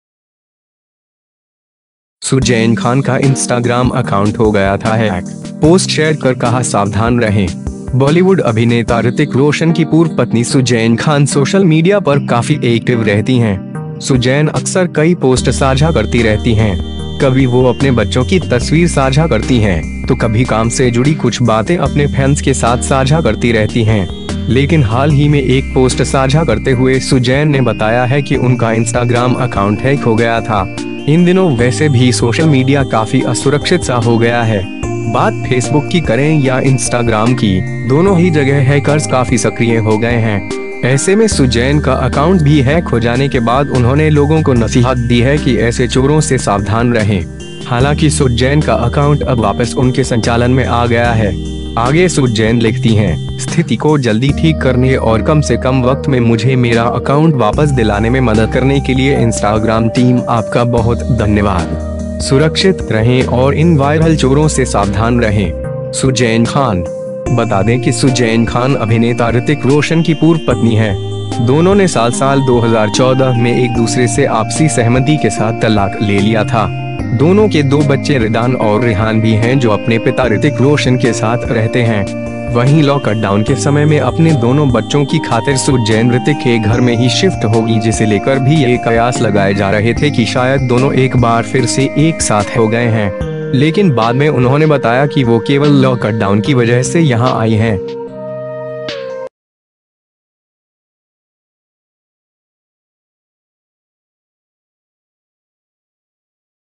सुजैन खान का इंस्टाग्राम अकाउंट हो गया था हैक, पोस्ट शेयर कर कहा 'सावधान रहें'
बॉलीवुड अभिनेता ऋतिक रोशन की पूर्व पत्नी सुजैन खान सोशल मीडिया पर काफी एक्टिव रहती हैं। सुजैन अक्सर कई पोस्ट साझा करती रहती हैं। कभी वो अपने बच्चों की तस्वीर साझा करती हैं, तो कभी काम से जुड़ी कुछ बातें अपने फैंस के साथ साझा करती रहती हैं। लेकिन हाल ही में एक पोस्ट साझा करते हुए सुजैन ने बताया है कि उनका इंस्टाग्राम अकाउंट हैक हो गया था।

इन दिनों वैसे भी सोशल मीडिया काफी असुरक्षित सा हो गया है। बात फेसबुक की करें या इंस्टाग्राम की, दोनों ही जगह हैकर्स काफी सक्रिय हो गए हैं। ऐसे में सुजैन का अकाउंट भी हैक हो जाने के बाद उन्होंने लोगों को नसीहत दी है कि ऐसे चोरों से सावधान रहें। हालांकि सुजैन का अकाउंट अब वापस उनके संचालन में आ गया है।

आगे सुजैन लिखती हैं, 'स्थिति को जल्दी ठीक करने और कम से कम वक्त में मुझे मेरा अकाउंट वापस दिलाने में मदद करने के लिए इंस्टाग्राम टीम आपका बहुत धन्यवाद... सुरक्षित रहें और इन वायरल चोरों से सावधान रहें। सुजैन खान।'

बता दें कि सुजैन खान अभिनेता ऋतिक रोशन की पूर्व पत्नी हैं। दोनों ने साल साल 2014 में एक दूसरे से आपसी सहमति के साथ तलाक ले लिया था। दोनों के दो बच्चे ऋदान और ऋहान भी हैं जो अपने पिता ऋतिक रोशन के साथ रहते हैं। वहीं लॉकडाउन के समय में अपने दोनों बच्चों की खातिर सुजैन ऋतिक के घर में ही शिफ्ट हो गईं जिसे लेकर भी ये कयास लगाए जा रहे थे कि शायद दोनों एक बार फिर से एकसाथ हो गए हैं। लेकिन बाद में उन्होंने बताया कि वो केवल लॉकडाउन की वजह से यहां आई हैं।